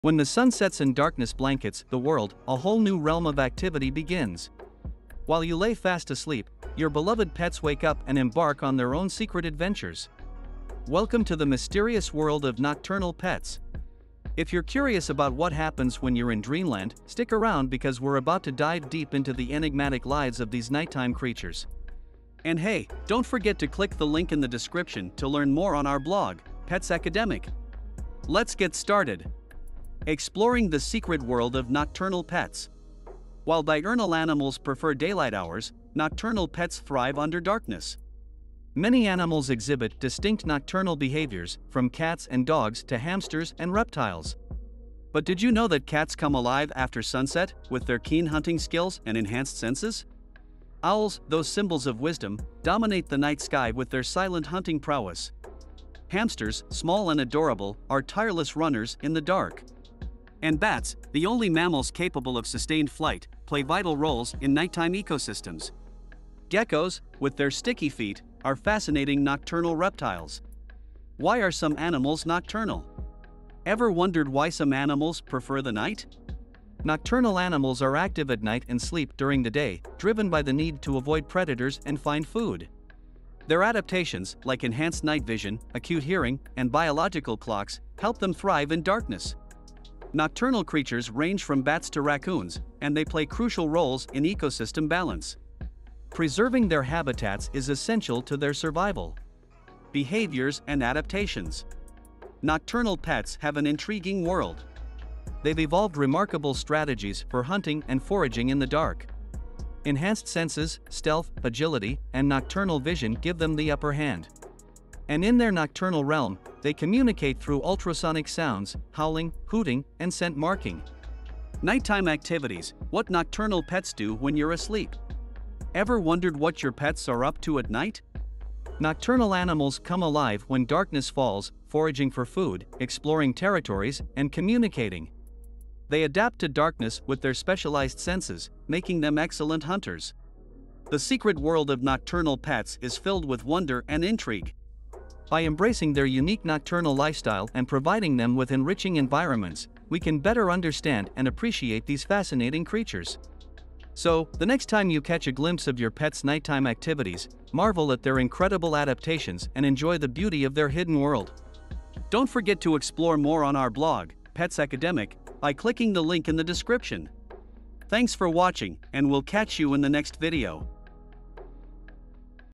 The Secret World Of Nocturnal Pets.
When the sun sets and darkness blankets the world, a whole new realm of activity begins. While you lay fast asleep, your beloved pets wake up and embark on their own secret adventures. Welcome to the mysterious world of nocturnal pets! If you're curious about what happens when you're in dreamland, stick around because we're about to dive deep into the enigmatic lives of these nighttime creatures. Let's get started!

Exploring The Secret World Of Nocturnal Pets

 While diurnal animals prefer daylight hours, nocturnal pets thrive under darkness. Many animals exhibit distinct nocturnal behaviors, from cats and dogs to hamsters and reptiles. But did you know that cats come alive after sunset, with their keen hunting skills and enhanced senses? Owls, those symbols of wisdom, dominate the night sky with their silent hunting prowess. Hamsters, small and adorable, are tireless runners in the dark. And bats, the only mammals capable of sustained flight, play vital roles in nighttime ecosystems. Geckos, with their sticky feet, are fascinating nocturnal reptiles.

Why Are Some Animals Nocturnal?
Ever wondered why some animals prefer the night? Nocturnal animals are active at night and sleep during the day, driven by the need to avoid predators and find food. Their adaptations, like enhanced night vision, acute hearing, and biological clocks, help them thrive in darkness. Nocturnal creatures range from bats to raccoons, and they play crucial roles in ecosystem balance. Preserving their habitats is essential to their survival.

Behaviors And Adaptations
 Nocturnal pets have an intriguing world. They've evolved remarkable strategies for hunting and foraging in the dark. Enhanced senses, stealth, agility, and nocturnal vision give them the upper hand. And in their nocturnal realm, they communicate through ultrasonic sounds, howling, hooting, and scent marking.

Nighttime Activities: What Nocturnal Pets Do When You're Asleep
 Ever wondered what your pets are up to at night? Nocturnal animals come alive when darkness falls, foraging for food, exploring territories, and communicating. They adapt to darkness with their specialized senses, making them excellent hunters.

The secret world of nocturnal pets is filled with wonder and intrigue. By embracing their unique nocturnal lifestyle and providing them with enriching environments, we can better understand and appreciate these fascinating creatures. So, the next time you catch a glimpse of your pet's nighttime activities, marvel at their incredible adaptations and enjoy the beauty of their hidden world.